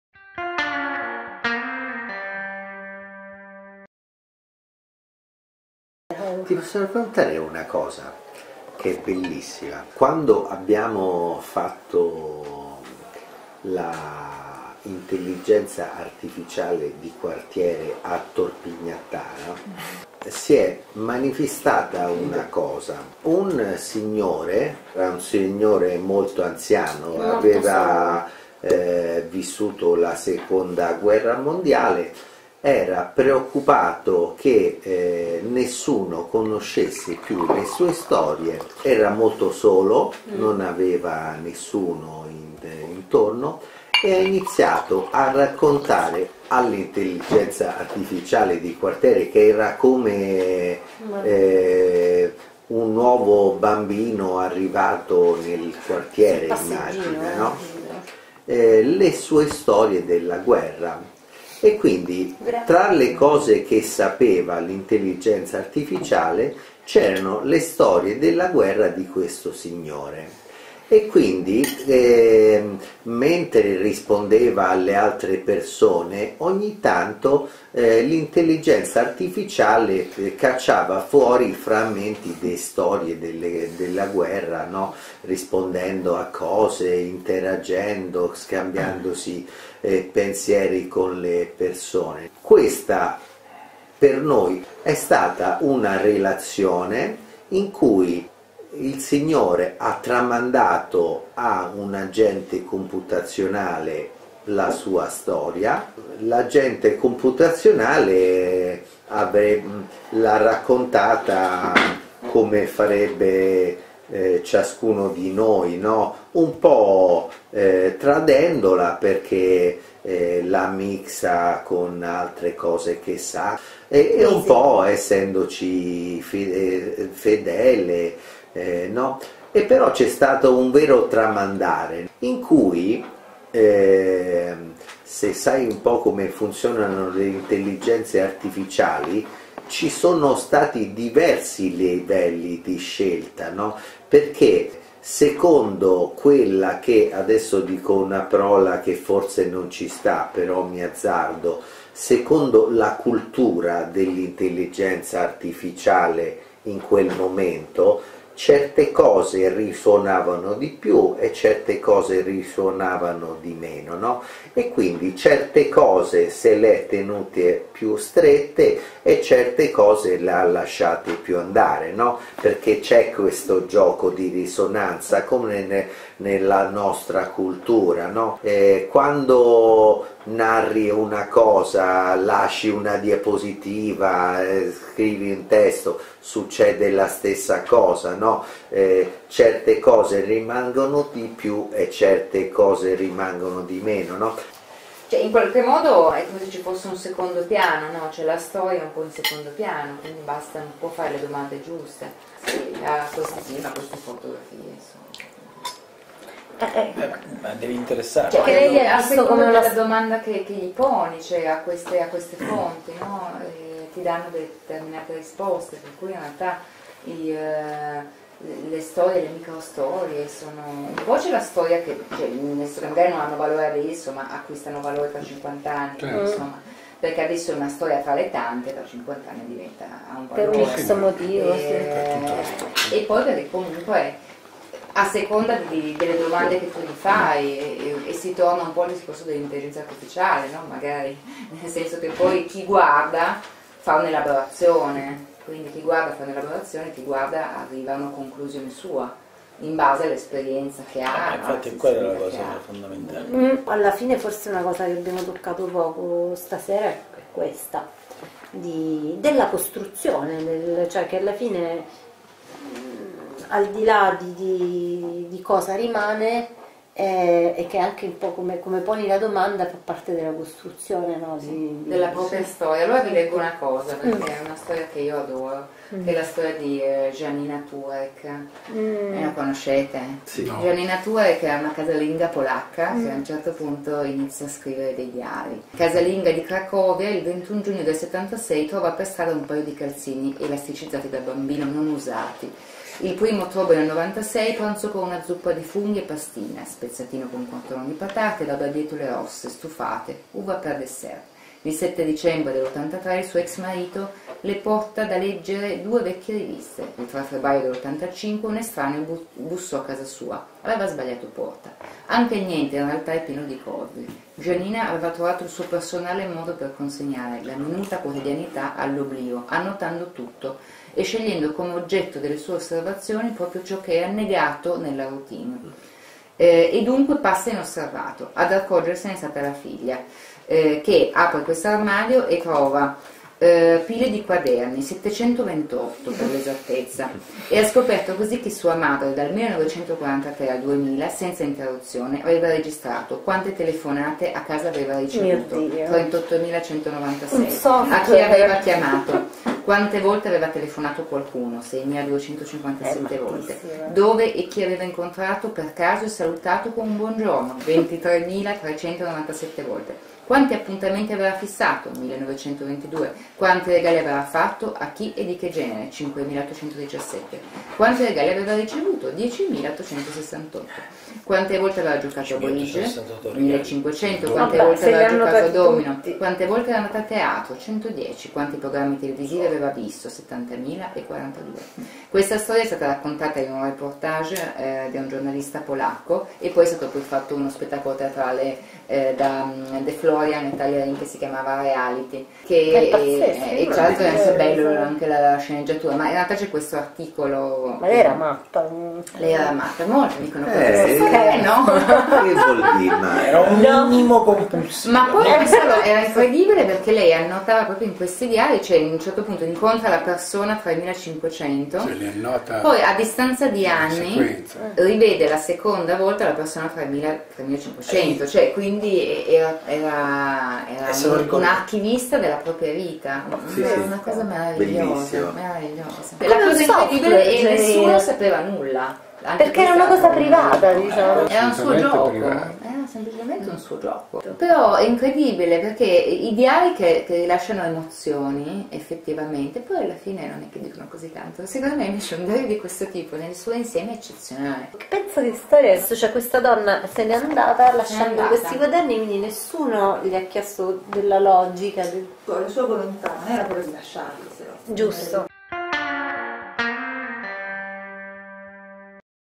COSTRUIRE MEMORIE: DUE SUPPORTI TECNICI PER DUE STORIE E LA STORIA
Alla fine ci siamo messi a raccontare storie. Storie di altri, storie minori: la storia di un vecchio di Torpignattara a Roma che racconta le sue avventure di guerra (la Seconda) a un’Intelligenza Artificiale e la storia di Janina Turek, una casalinga polacca, che trascrive il minuzioso accadere degli eventi della sua vita quotidiana (dal 1943 al 2000) su pagine di quaderni.

Un lascito di memorie, affidate a supporti tecnici che appartengono a momenti di civiltà diversi, la civiltà cartacea della scrittura e quella elettronica dell’oralità, due diversi modi di costruzione della memoria.

La storia tramandata ci interroga. Ma sono le nostre domande a interrogare la storia, a fare luce sulla rilevanza di documenti, le cui tracce nel tempo sono state relegate in secondo piano. È questo interrogare a partire da soggetti marginali, e magari sconfitti dalla storia, a consentirci di apprendere una storia diversa.

Un’intelligenza artificiale, con i suoi diversi livelli di apprendimento, può aiutarci in questa esplorazione?

(6, Fine)